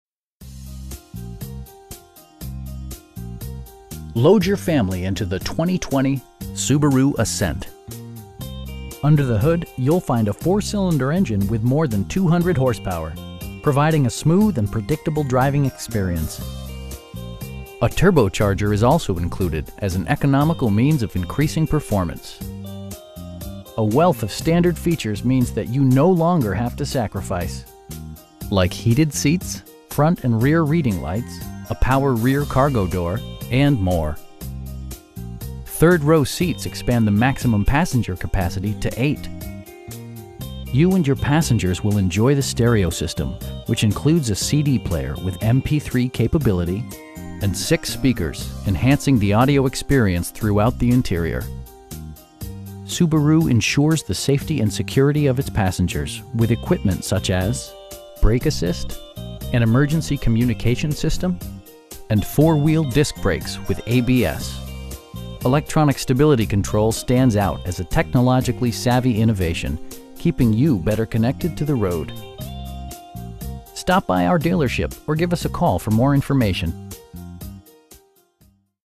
2020 Subaru Ascent Premium 8-Passenger
Load your family into the 2020 Subaru Ascent. Under the hood you'll find a 4 cylinder engine with more than 200 horsepower, providing a smooth and predictable driving experience. A turbocharger is also included as an economical means of increasing performance. A wealth of standard features means that you no longer have to sacrifice. Like heated seats, front and rear reading lights, air conditioning, power door mirrors, a power rear cargo door, lane departure warning, cruise control, and more. Third row seats expand the maximum passenger capacity to 8. For drivers who enjoy the natural environment, a power moon roof allows an infusion of fresh air. You and your passengers will enjoy the stereo system, which includes a CD player with MP3 capability, steering wheel mounted audio controls, and 6 speakers, enhancing the audio experience throughout the interior. Subaru ensures the safety and security of its passengers with equipment such as: dual front impact airbags, head curtain airbags, traction control, brake assist, a panic alarm, an emergency communication system, and 4 wheel disc brakes with ABS. Electronic stability control stands out as a technologically savvy innovation, keeping you better connected to the road. You will have a pleasant shopping experience that is fun, informative, and never high pressured. Stop by our dealership or give us a call for more information.